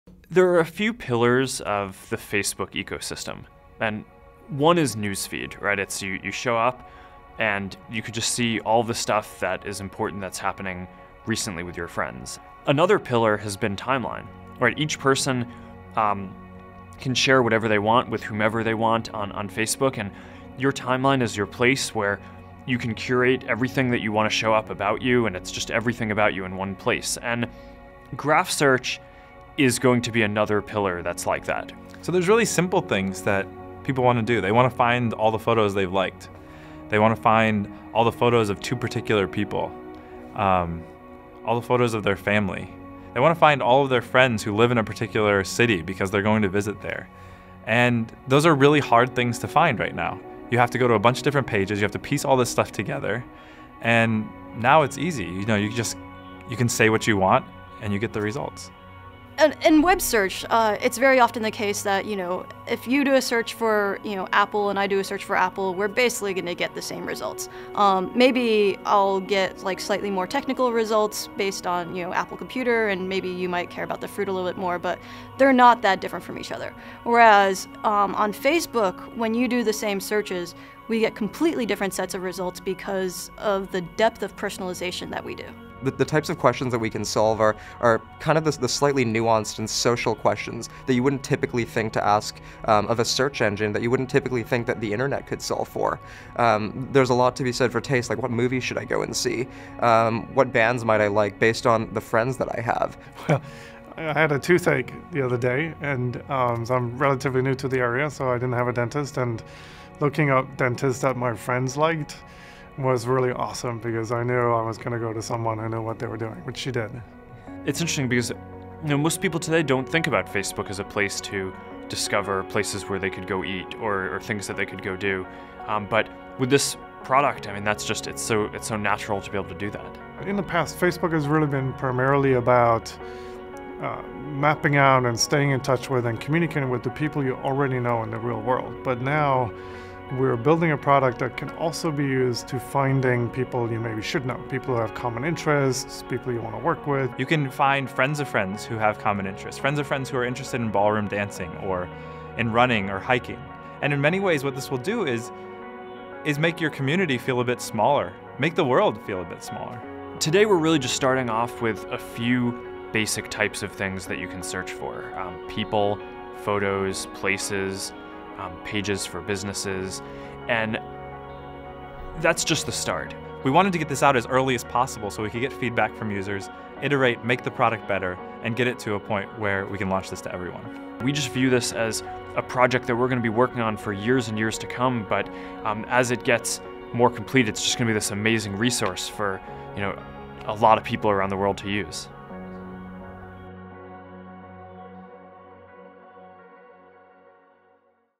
Mark Zuckerberg explains Facebook's new Graph Search
In this video provided by Facebook, CEO Mark Zuckerberg and other Facebook employees discuss the company's new Graph Search technology. The company announced the beta of the new feature on Jan. 15, 2013.